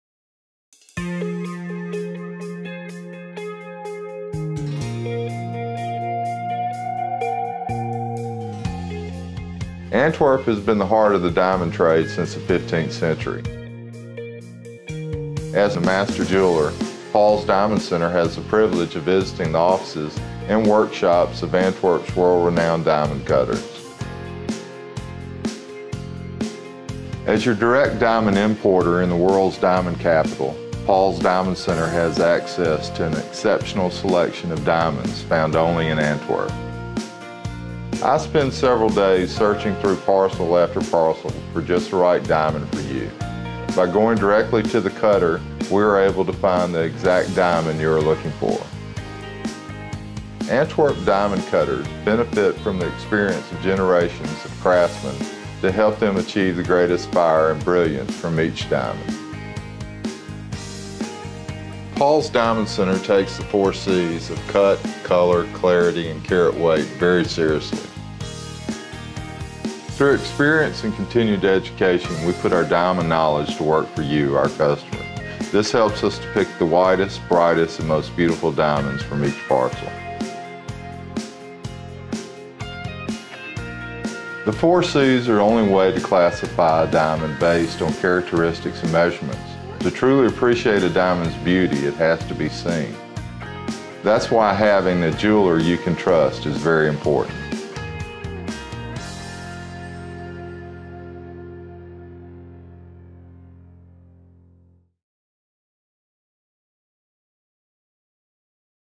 Paul's Diamond Center Antwerp,Belgium
We travel to Antwerp Belgium. And hand pick only the best diamonds from the diamond cutters for our customers.